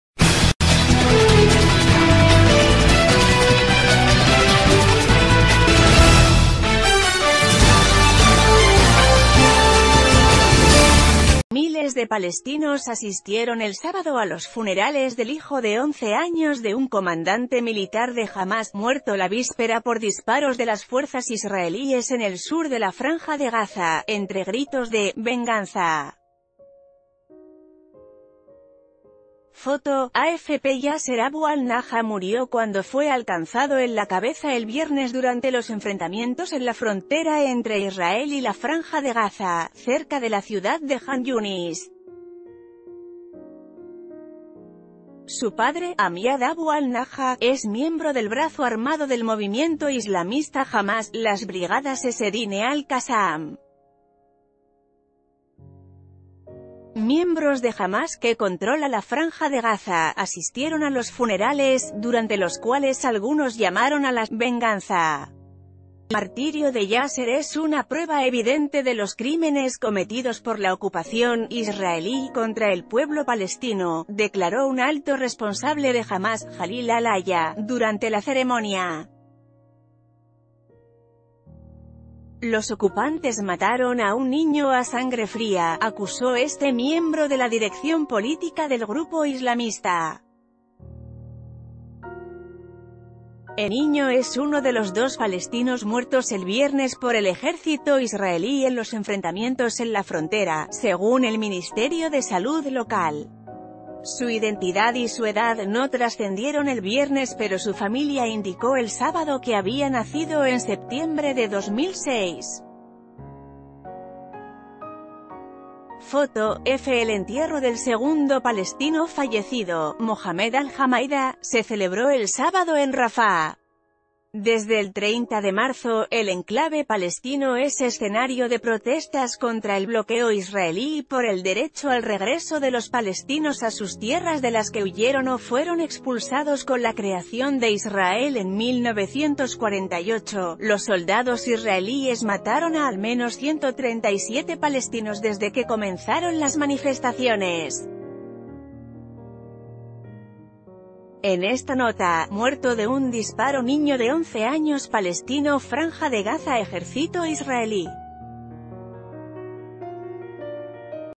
"¡Venganza!" exigen entre gritos por el niño palestino asesinado
Miles de palestinos asistieron el sábado a los funerales del hijo de 11 años de un comandante militar de Hamas, muerto la víspera por disparos de las fuerzas israelíes en el sur de la Franja de Gaza, entre gritos de "venganza". Foto: AFP Yaser Abu Al Naja murió cuando fue alcanzado en la cabeza el v...